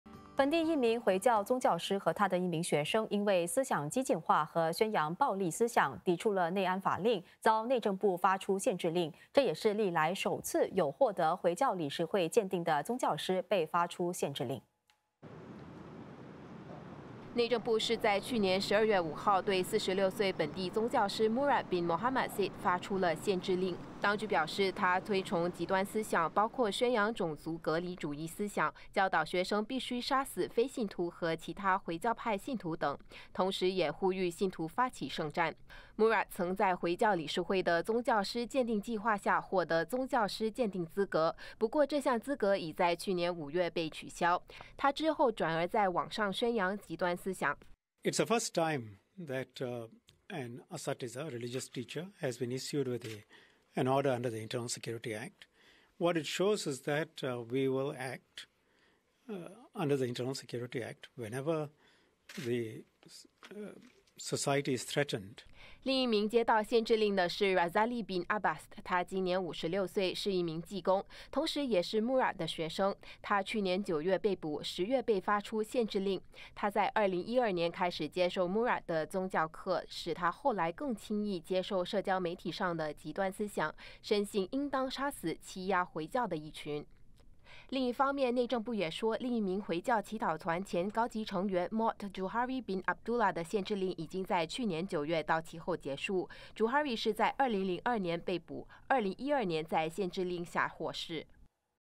内政部对两人发出内安法限制令 其中一名是回教导师
内政部对两人发出内安法限制令 其中一名是回教导师 内政部在内部安全法令（ISA）下对两人发出限制令（restriction order），其中一人是回教导师，另一人是他的前学生。这是我国第一次对回教导师发出限制令。